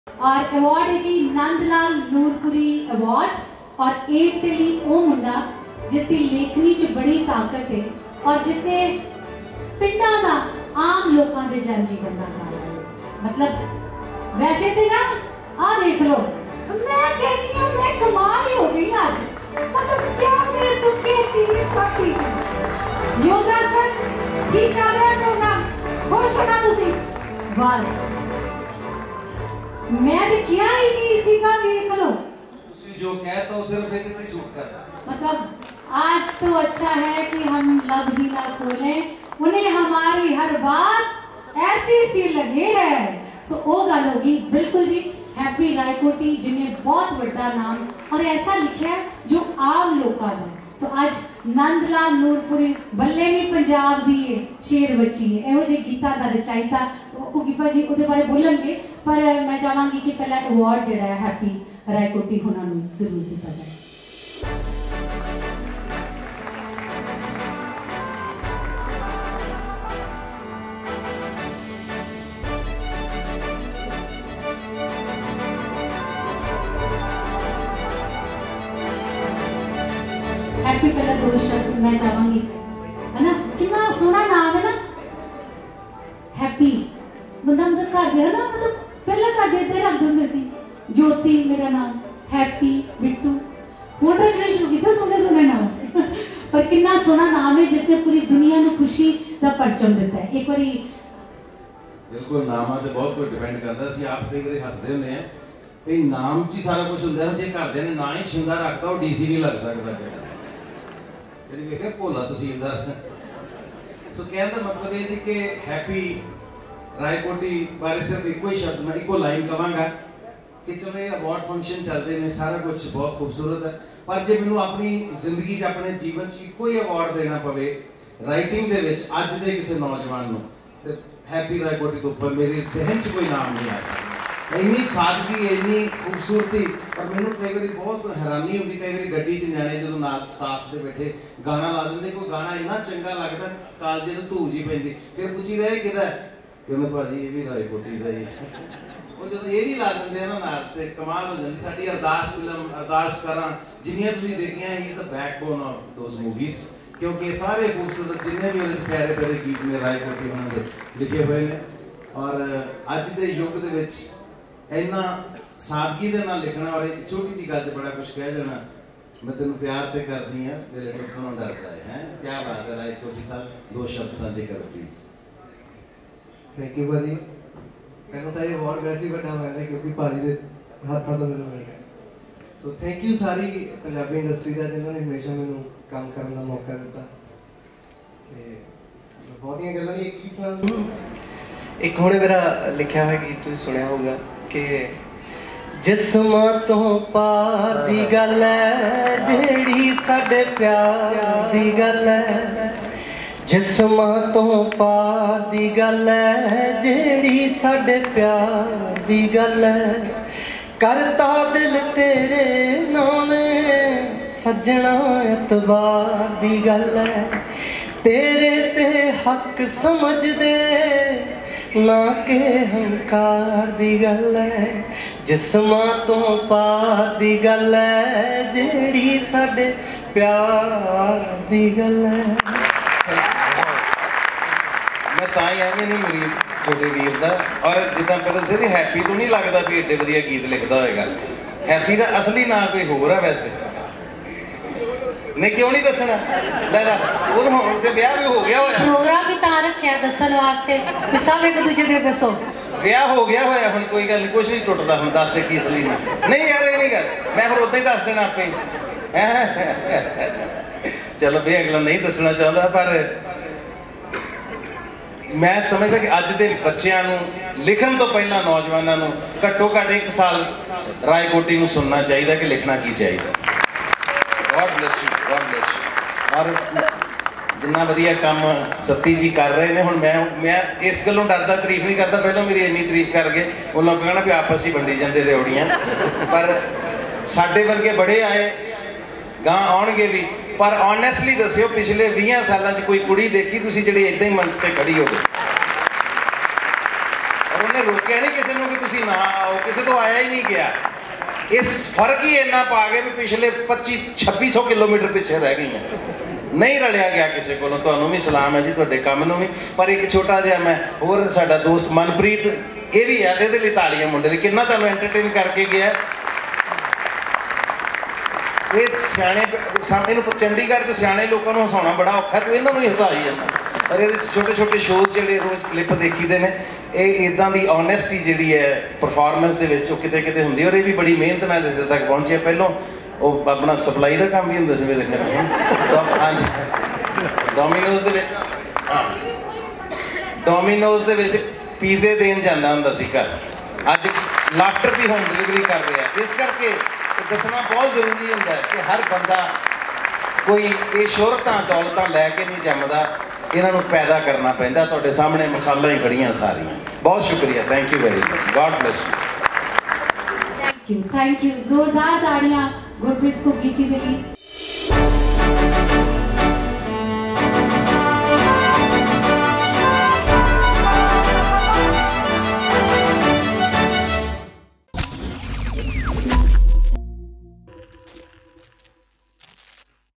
Happy Raikoti honoured with “Jaswant Bhanwra Memorial” Award | Pefa 2023 | Gurpreet Ghuggi
It's a Fivewood Media Presentation.